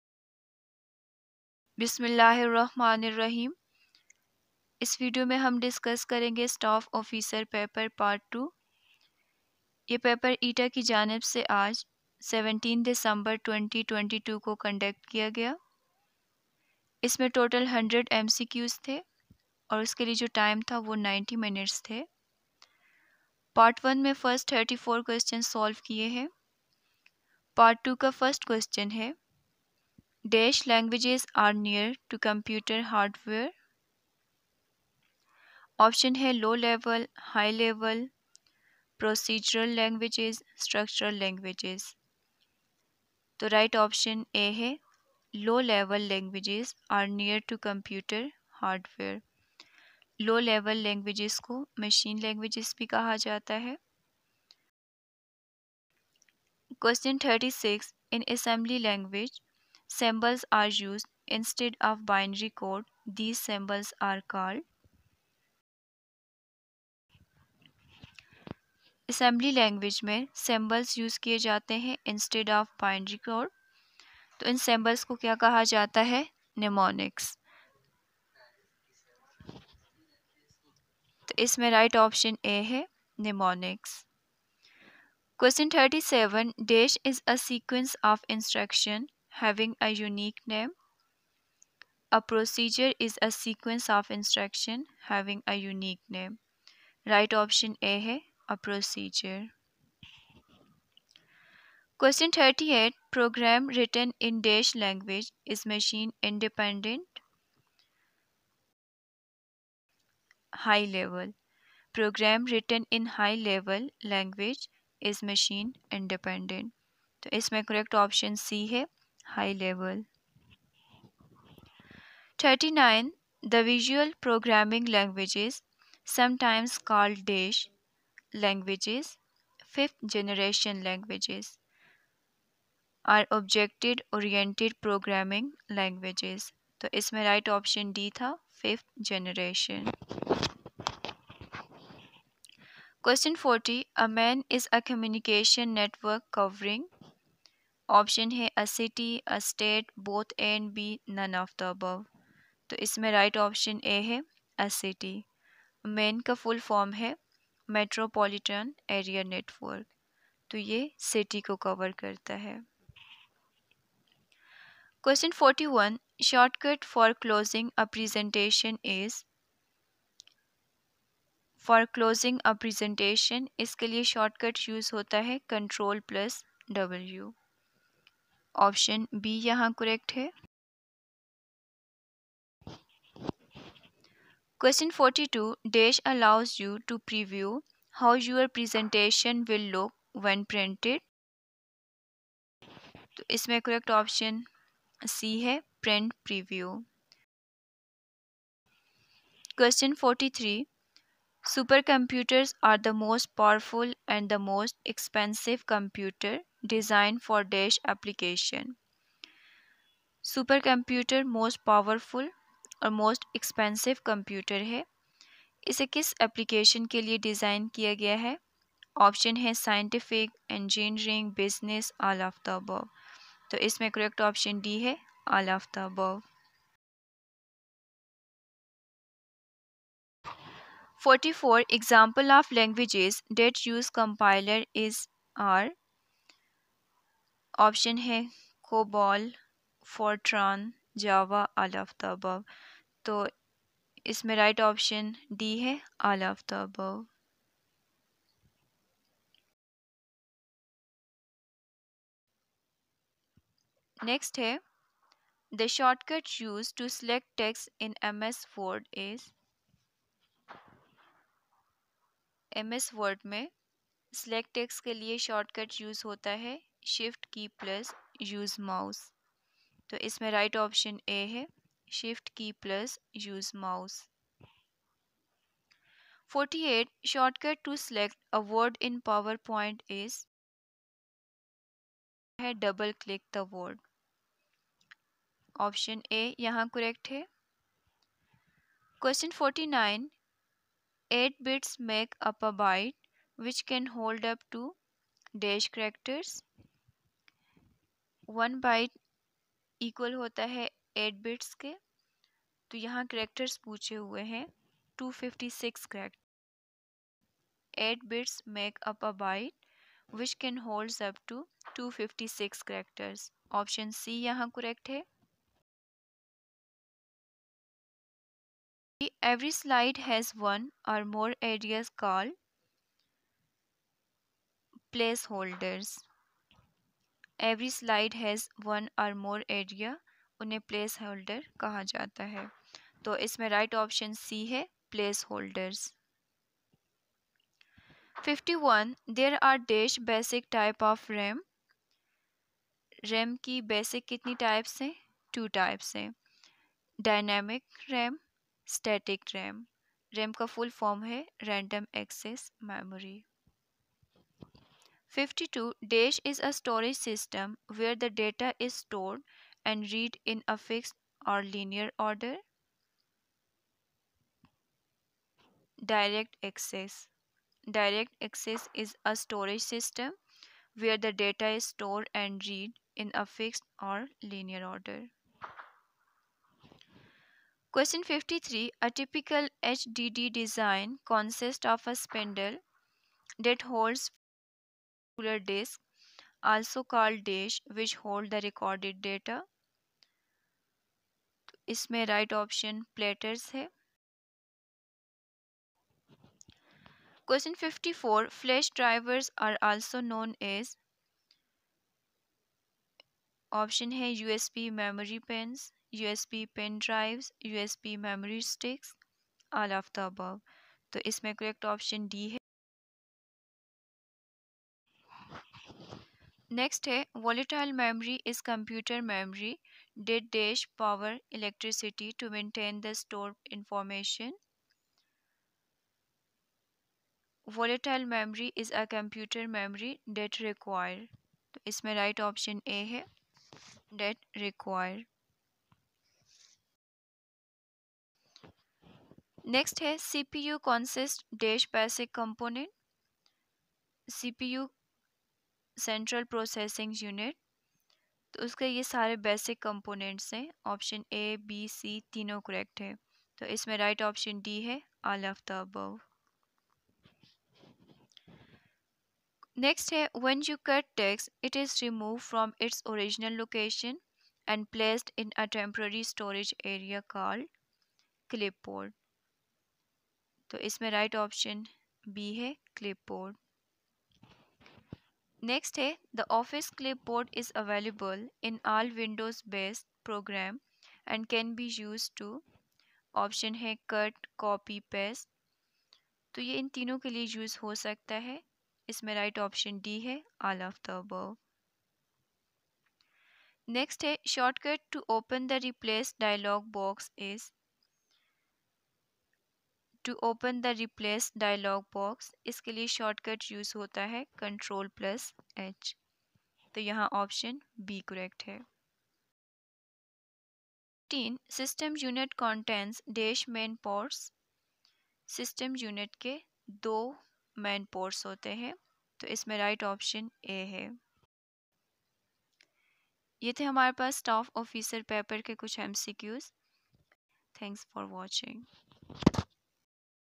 ETEA Staff Officer BPS-14 Solved Paper : Answer keys : Test Date : 17 December 2022
ETEA Staff Officer BPS-14 Solved Paper held on 17 December 2022  : Public Sector Organization Complete solved paper